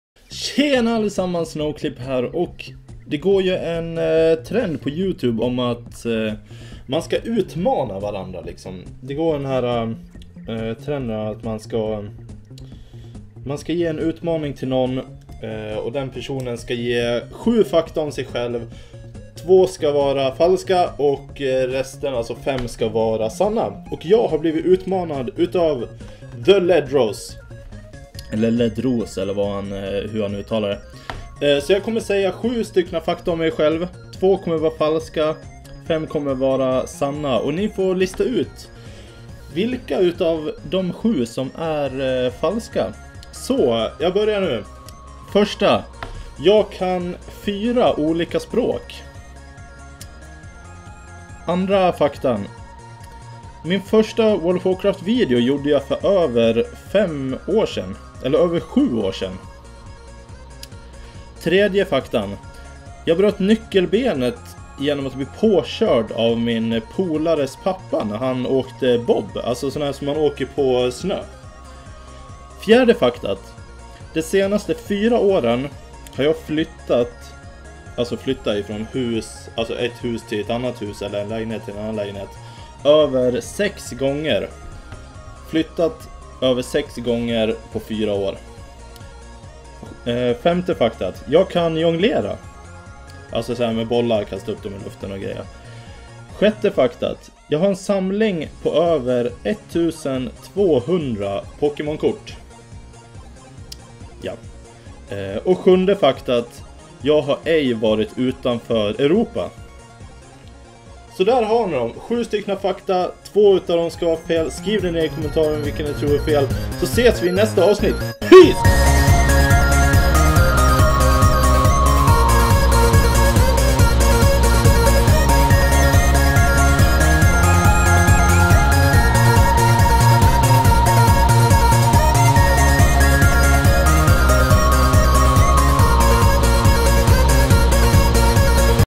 Utmanad av TheLedros - 7 fakta, 2 falska
Blev utmanad av TheLedros att göra en video där jag skulle säga 7 fakta om migsjälv, 5 sanna och 2 falska! Skriv i kommentarsfältet vilka ni tror är de falska! :)